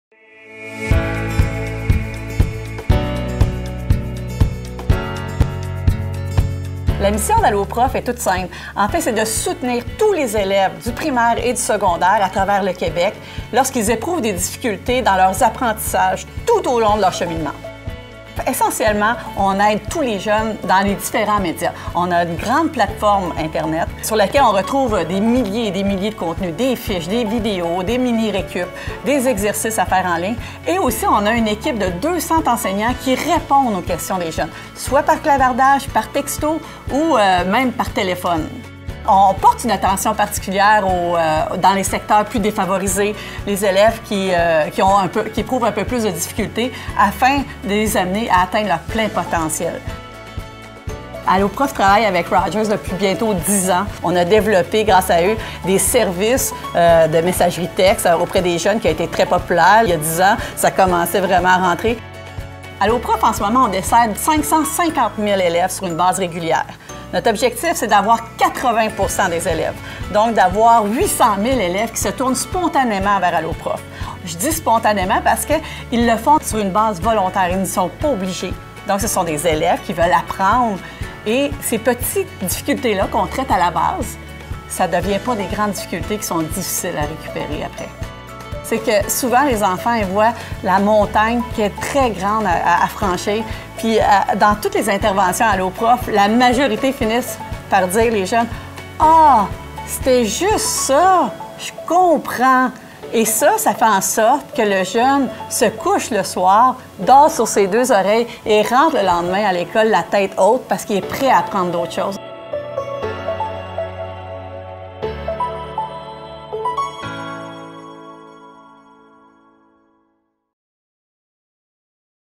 Ted Rogers Community Grants: Alloprof
We’re proud to enable the next generation of Montreal leaders to reach their full potential. Here’s how Alloprof has been able to serve over 550,000 students in their educational journeys through resources made possible by the Ted Rogers Community Grants!